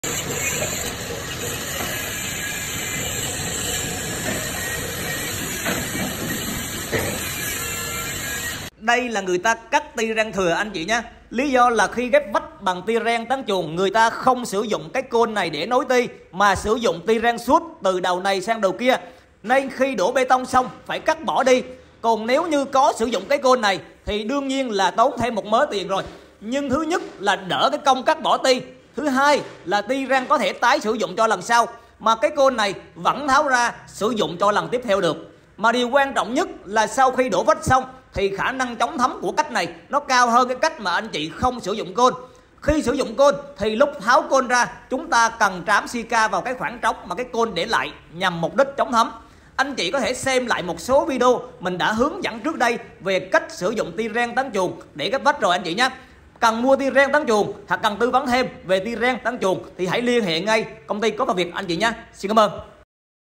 Có Và Không Sử Dụng Côn Chống Thấm Khi Ghép Vách Bằng Tyren Tán Chuồn Sẽ Ra Sao | CỐP PHA VIỆT
Đây là người ta đang cắt ty ren thừa anh chị nhé, lý do là khi ghép vách bằng tyren tán chuồn người ta không sử dụng cái côn này để nối ty, mà sử dụng ty ren suốt từ đầu này sang đầu kia, nên sau khi đổ bê tông xong phải cắt bỏ, còn nếu như có sử dụng cái côn này, thì đương nhiên là tốn thêm 1 mớ tiền, nhưng thứ 1 là đỡ công cắt bỏ ty, thứ 2 là ty ren có thể tái sử dụng cho lần sau, mà cái côn này vẫn tháo ra sử dụng tiếp cho lần sau được, mà điều quan trọng nhất là sau khi đổ vách xong thì khả năng chống thấm của vách cao hơn việc không sử dụng côn, khi sử dụng côn thì lúc tháo côn chúng ta cần trám sika vào khoản trống mà cái côn để lại nhằm mục đích chống thấm. Anh chị có thể xem lại một số video hướng dẫn sử dụng tyren tán chuồn ghép vách mà mình đã hướng dẫn ở các video trước.
Xem thêm video khác
CÔNG TY TNHH PHỤ KIỆN VÀ CỐP PHA VIỆT
– Địa chỉ : 19A Nguyễn Văn Bứa, Xuân Thới Sơn, Hóc Môn, Thành phố Hồ Chí Minh.